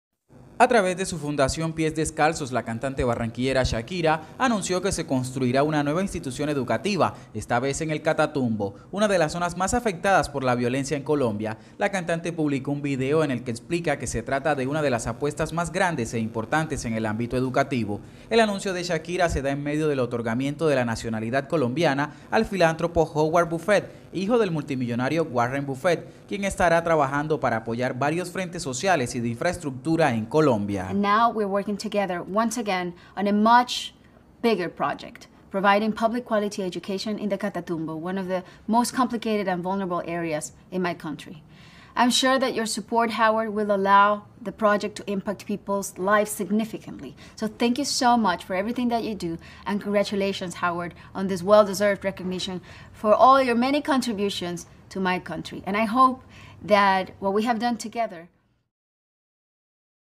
Shakira anunció que construirá un mega colegio en el Catatumbo
Artistas| A través de su Fundación Pies Descalzos, la cantante barranquillera Shakira anunció que se construirá una nueva institución educativa, esta vez en el Catatumbo, una de las zonas más afectadas por las violencia en Colombia. La cantante publicó un video en el que explica que se trata de una de las apuestas más grandes e importantes en el ámbito educativo.

'Siempre he creído en el poder del hacer y lo que podemos hacer trabajando juntos', dijo la artista, quien agradeció el apoyo incondicional del magnate y filántropo  Howard G. Buffett. 'Es por eso que hace más de una década decidí contactar a este hombre de negocios que resultó ser un gran ser humano con un gran corazón'.

'Él, sin saber mucho de mi país le apostó a la Fundación Pies Descalzos, que mejora el presente y el futuro de los niños a través de la educación', agregó Shakira, quien hace varios años también entregó dos planteles educativos, uno en Barranquilla y otro en Cartagena.

'Estoy segura que tu ayuda Howard permitirá que el proyecto (de el Catatumbo) impacte a personas significativamente. Gracias por todo lo que has hecho y felicitaciones Howard por este merecido reconocimiento por tu colaboración.', recalcó.

El anuncio de Shakira se da en medio del otorgamiento de la nacionalidad colombiana al filántropo Howard G. Buffett, hijo del multimillonario Warren Buffett, quien estaría trabajando para apoyar varios frentes sociales y de infraestructura en Colombia.